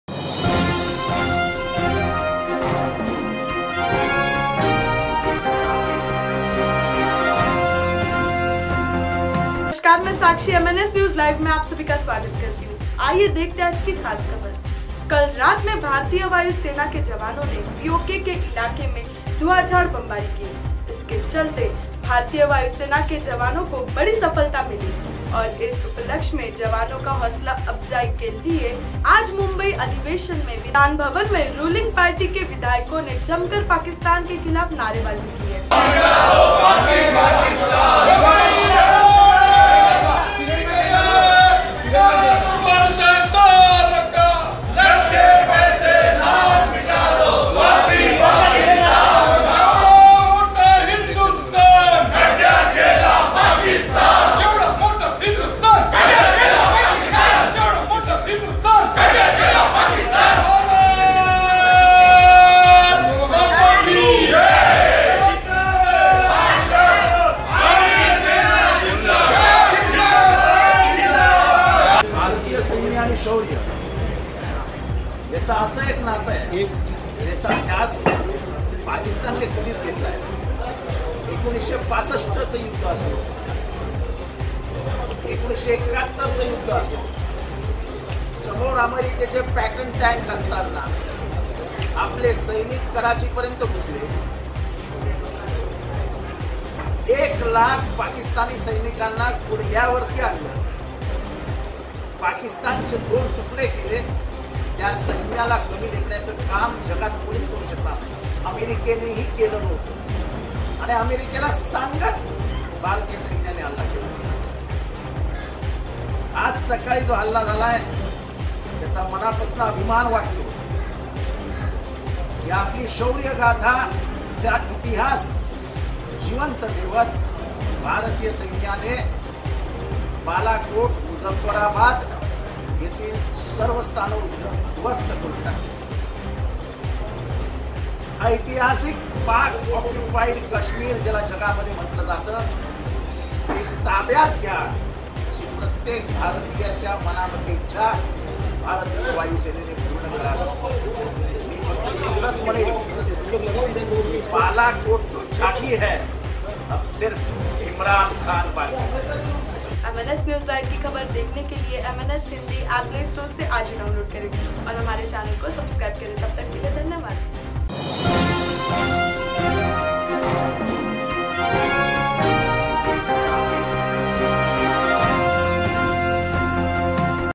MNS NEWS LIVE IN HINDI मुंबई विधान भवन में पाकिस्तान के खिलाफ जमकर हुई नारे बाजी
MNS NEWS LIVE IN HINDI मुंबई अधिवेशन में हुआ भारतीय  वायुसेना  का सम्मान
जानो  जीतेन्द्र आव्हाड का मत
जीतेन्द्र आव्हाड